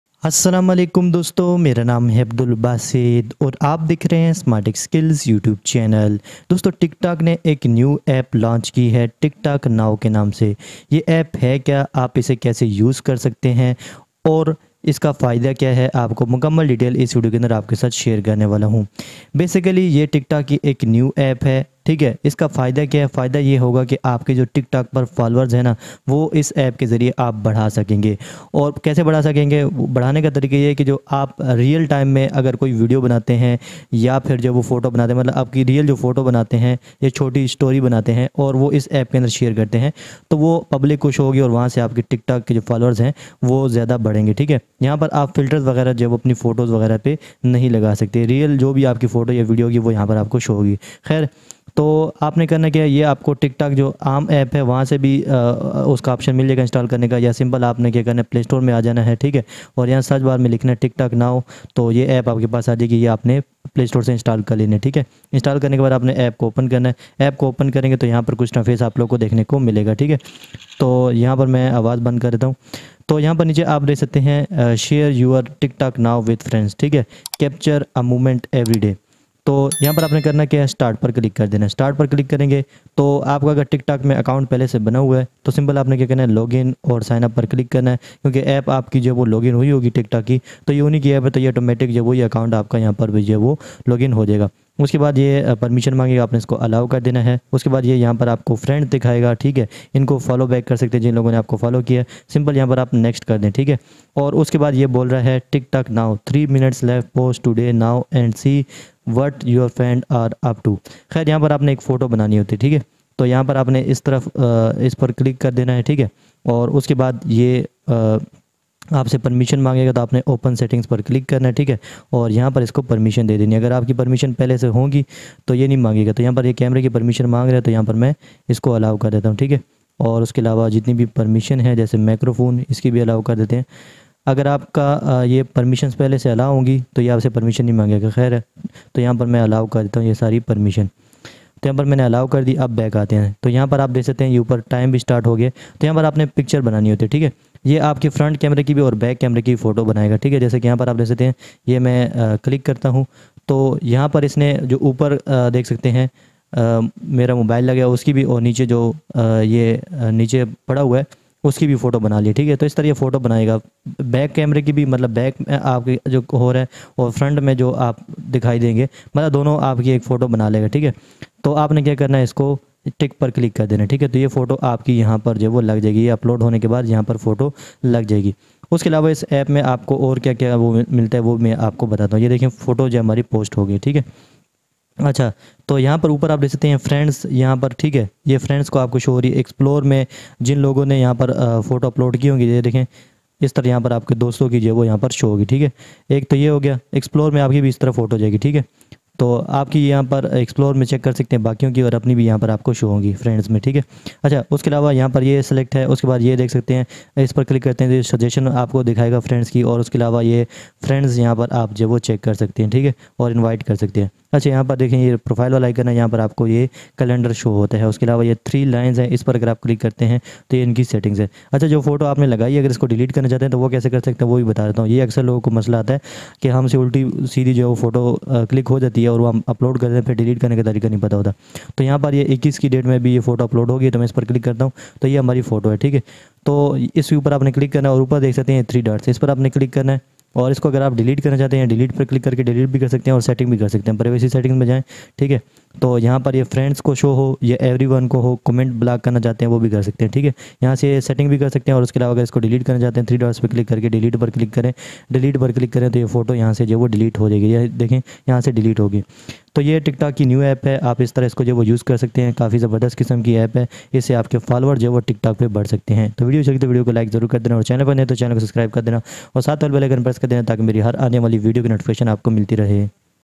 How to Use Tiktok Now App | Tiktok Now App Use karne ka tarika | Tiktok Now App kaise use kare
Tiktok Now App Tiktok ki new app hai as video mai bataya hai mai ne ki tiktok now app kya hai or ap tiktok now app kaise use kar sakte hain